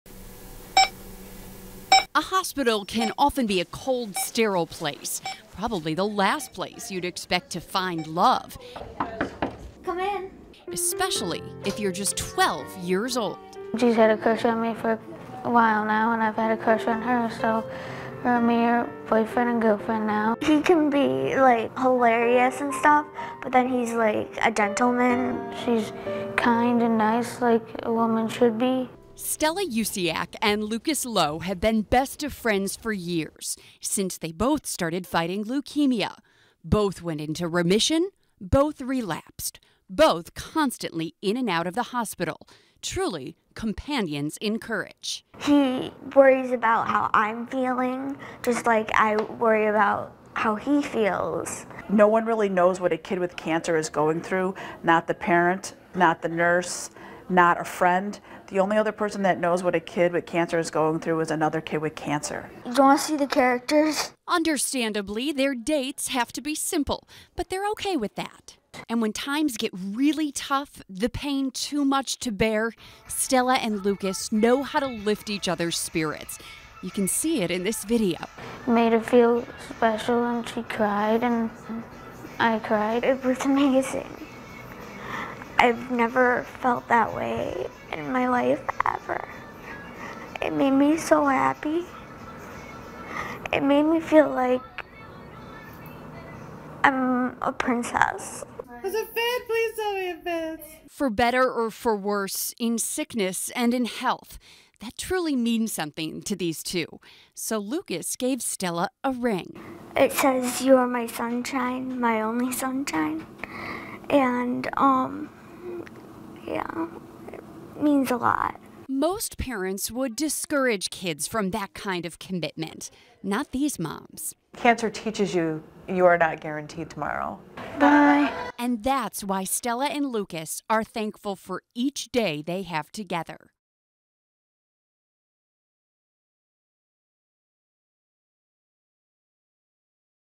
12-year-olds fall in love while fighting cancer
A hospital can often be a cold, sterile place, and probably the last place you'd expect to find love. That's especially true if you're just 12-years-old. But Stella Usiak and Lucas Lowe have found something special.

Melissa Holmes, WGRZ-TV, Buffalo-
BUFFALO - A hospital can often be a cold, sterile place, and probably the last place you'd expect to find love. That's especially true if you're just 12-years-old. But Stella Usiak and Lucas Lowe have found a very special friendship while both fighting a very serious disease.

"She's had a crush on me for a while now, and I've had a crush on her, so her and me are boyfriend and girlfriend now," said Lucas.

Stella describes Lucas as hilarious and "a gentleman." And Lucas describes Stella as kind and nice, "like a woman should be," he said.

Lucas and Stella first met when they both started fighting Acute Lymphoblastic Leukemia. Both went into remission, then both relapsed. Both had bone marrow transplants and both have constantly been in and out of hospitals, including Roswell Park Cancer Institute.

"He worries about how I'm feeling just like I worry about how he feels," said Stella.

The hospital staff has watched their special friendship grow.

"No one really knows what a kid with cancer is going through- not the parent, not the nurse, not a friend- the only other person that knows what a kid with cancer is going through is another kid with cancer," said Roswell Park Registered Nurse Angelica Zachara.

Understandably, Lucas and Stella's dates have to be simple, and they're ok with that. They have lunch, Lucas pushes Stella in her wheelchair, and they just talk.

"It was like this cute little old couple, sitting on the porch. It's very rare that kids their age could kind of have that sort of compassion for each other," said Stella's mom, Jennifer Usiak.

They say mostly their conversations do not revolve around their disease and treatment. They just chat about normal kid stuff. While they are mature and different from most kids, they're young and innocent, too.

"She just wants a boy to like her. She just wants to have somebody make her feel special," said Stella's mom.